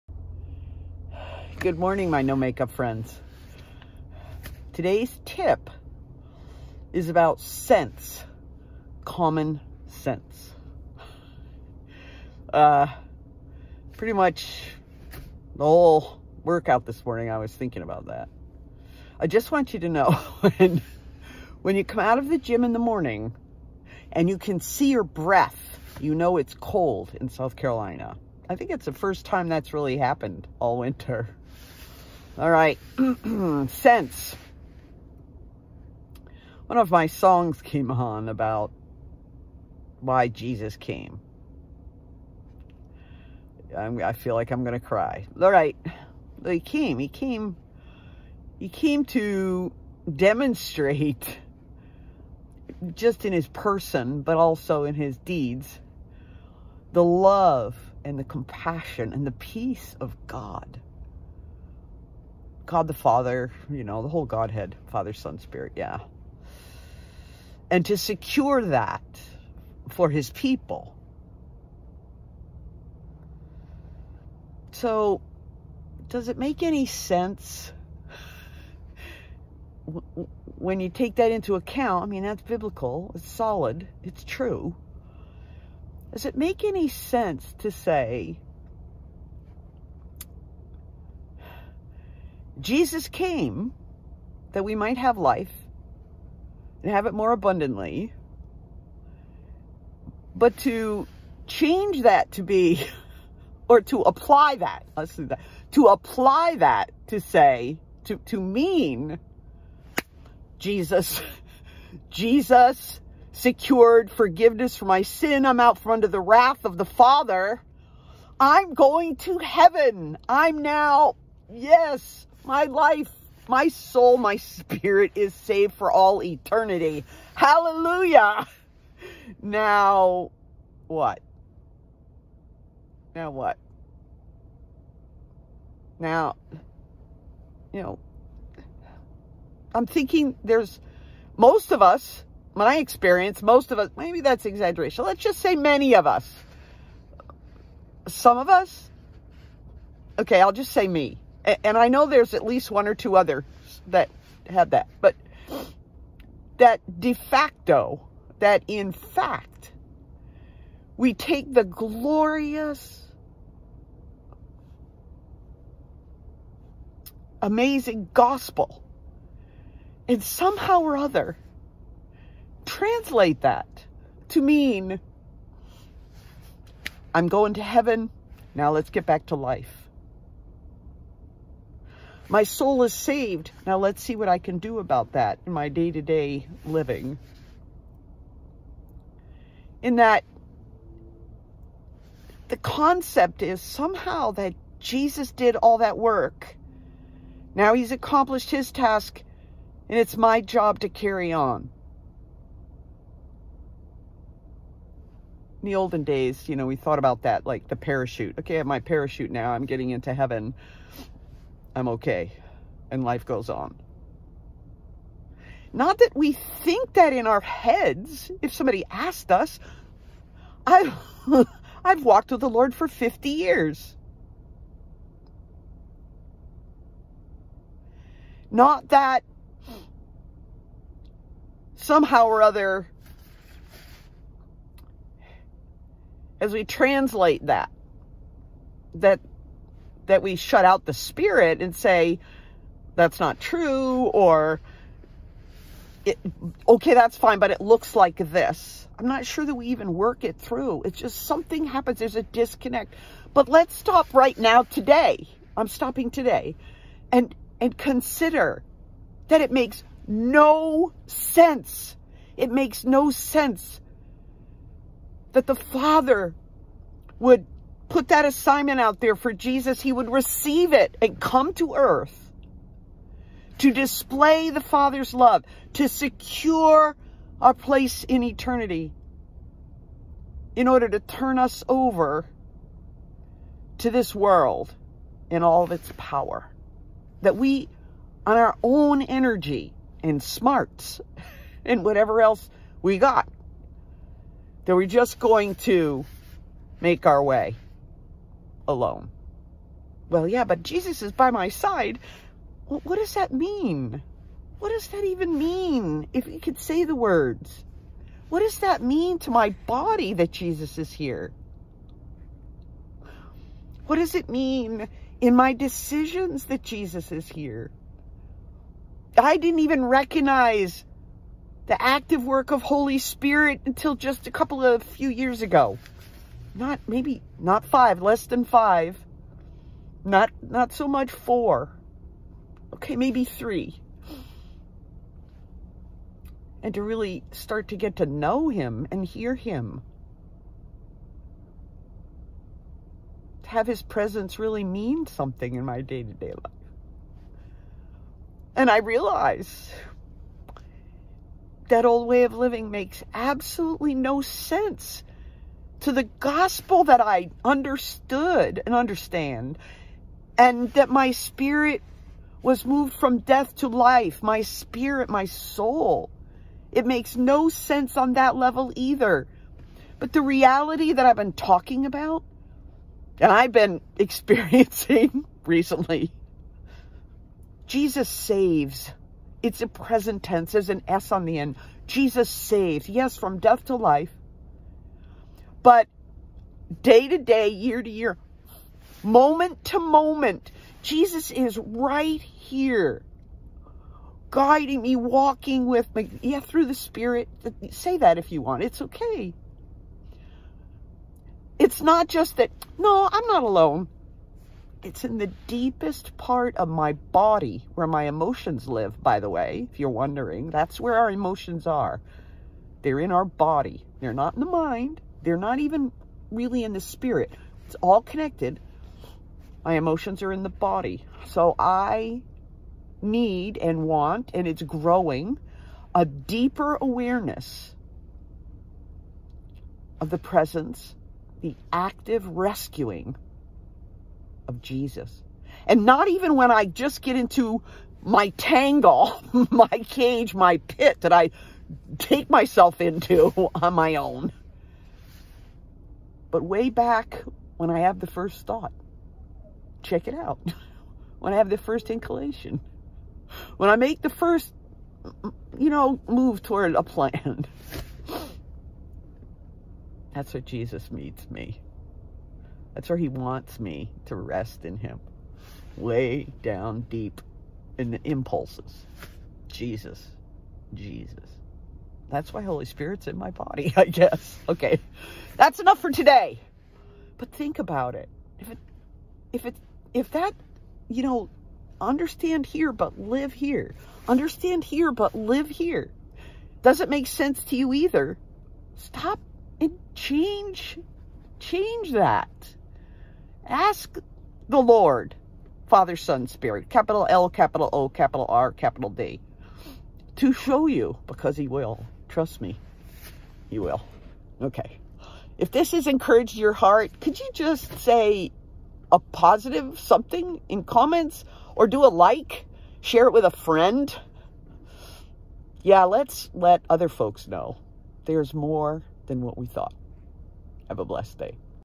TIP: Common Sense, The Gospel, and Tears 2 23 21
Let's consider the Gospel with a bit of common sense, and practice the conclusions in everyday life.